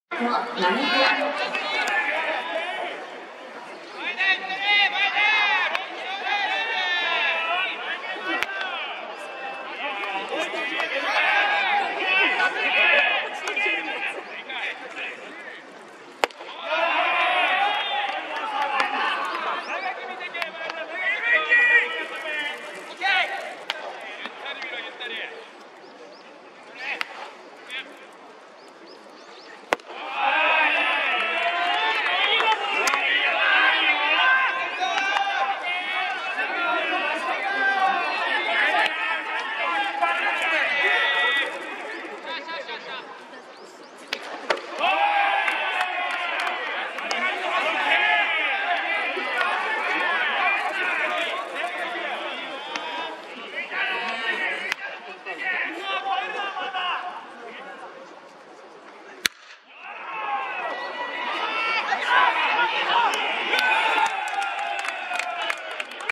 前田 一輝（TDK）No.2 スポニチ大会 VS王子 2023/3/6
前田　一輝（鳴門）

【鳴門】では１年秋には主戦投手で四国大会に出場。
２年秋から外野手に転向し、四国大会準優勝。
３年春の選抜の初戦では優勝した大阪桐蔭と対戦し、４番として安打も放ったが、チームは惜敗。
３年夏も甲子園に出場し、初戦で選抜準優勝した近江と対戦。
山田（西武）から先制となる三塁打を放ったが、チームは敗れた。
また、途中からリリーフとしてもマウンドに上がった。

【TDK】では初の公式戦となったスポニチ大会では３試合に出場するが、打率.143と悔しい結果となったが、守備ではファインプレーがあるなど、対応力に高さを魅せた。